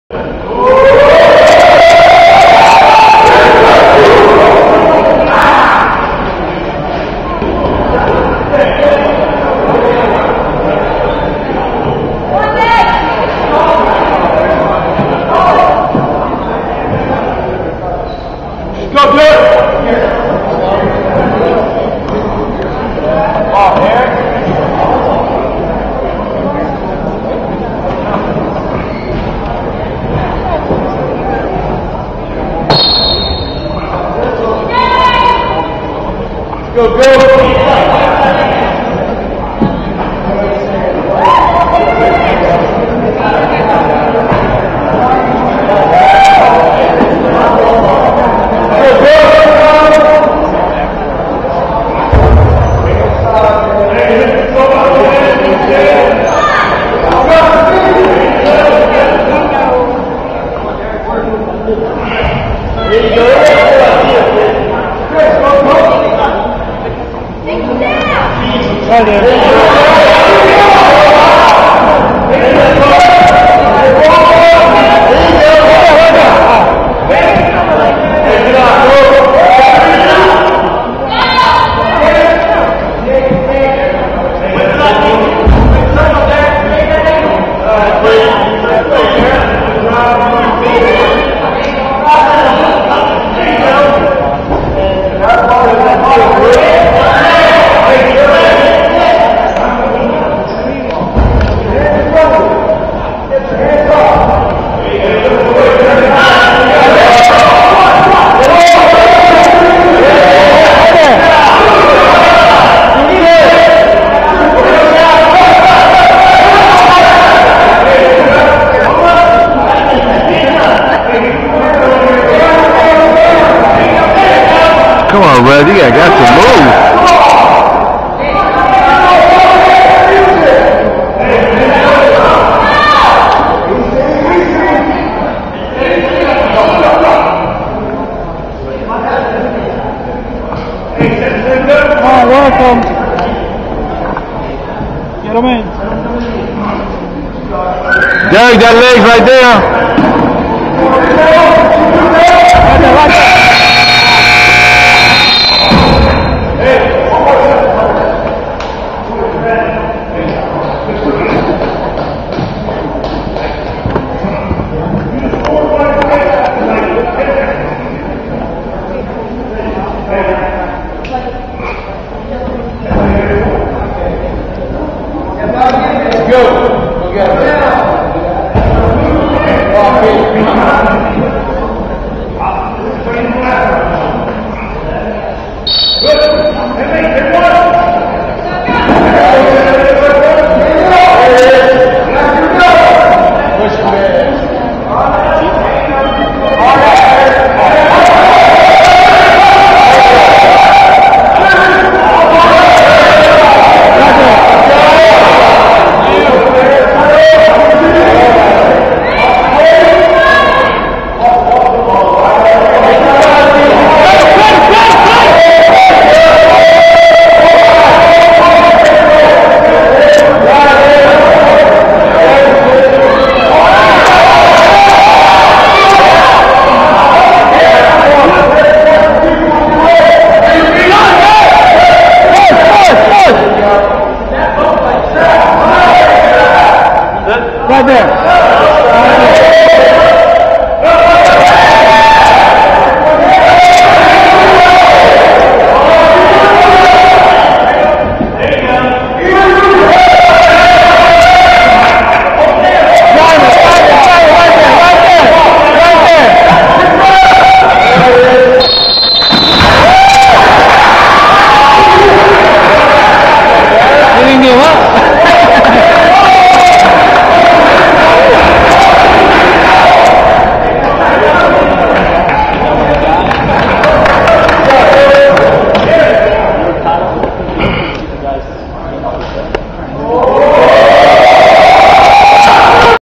Derik Gotay pins Seminole H.S. wrestler on 1-12-10
Derik Gotay pins Seminole H.S. wrestler @ The Lk. Howell Duals on 1/12/2010